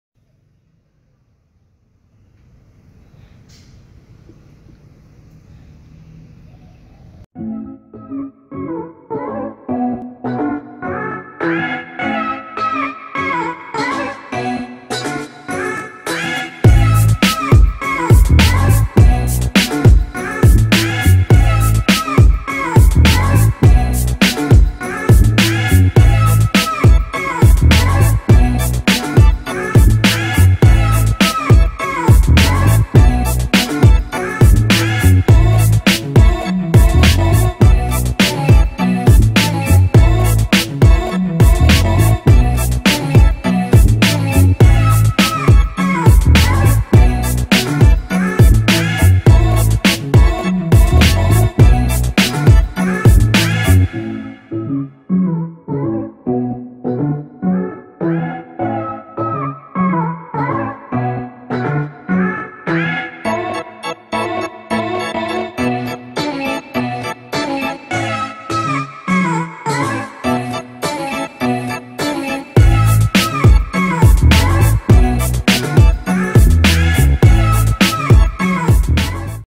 How I try to repair my deep scratch on Saga FLX||Shopee Shopping: Car Repair compound
Last year  I had an accident, This accident created deep scratch on my rear bumper. I know gotta change the whole bumper or just make touch up by car specialists.

But here I am, bump into a product that visualise a deep scratch is repairable. How the outcome turn out to be?